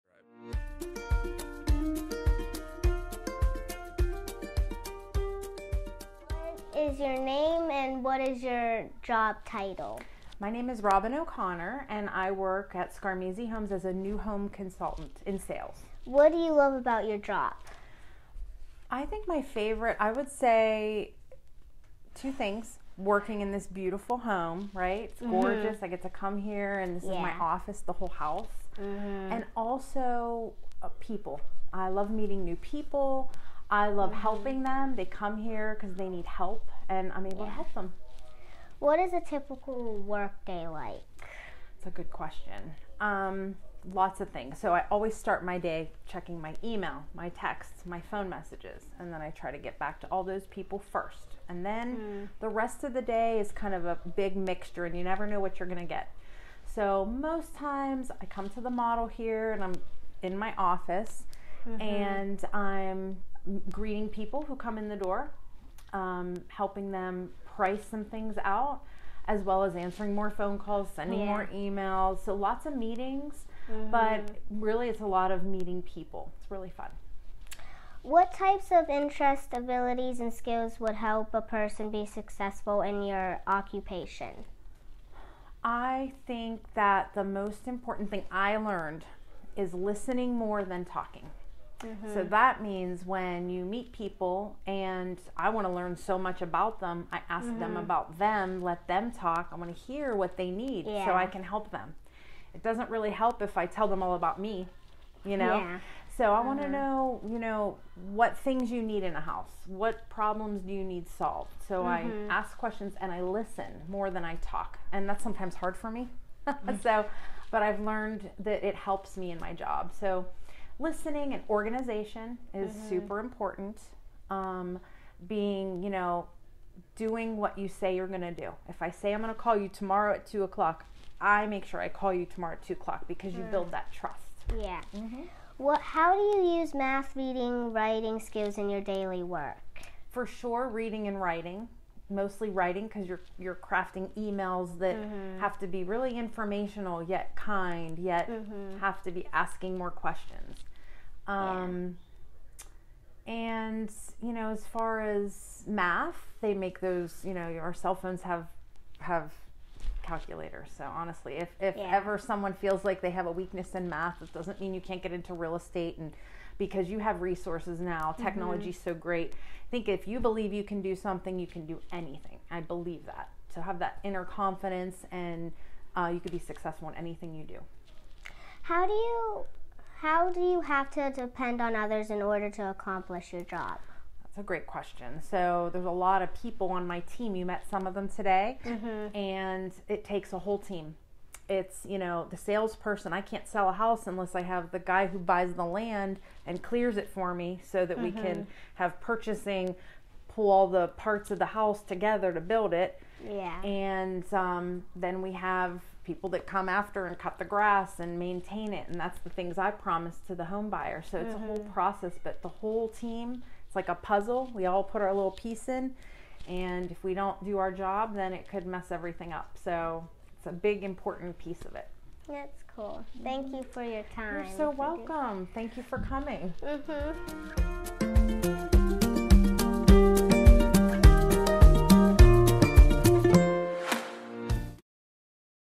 Sales Consultant: Meet Robin! | World of Work Career Map
Meet Robin! She is a New Home Consultant in Sales at Scarmazzi Homes. Let's join our student reporters as they discover various careers in their community!

For more information about WQED Education's World of Work and the Ready to Learn Learning Neighborhood you can

Students from Canon-McMillan School District in Canonsburg, PA